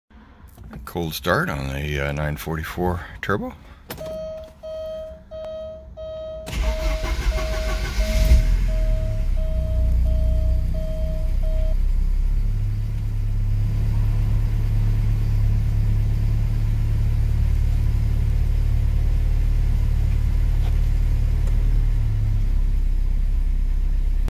Porsche 944 Turbo Cold Start
Overnight cold start on a stock 1987 944T. No check engine lamp. Autobahn1, Wichita, KS.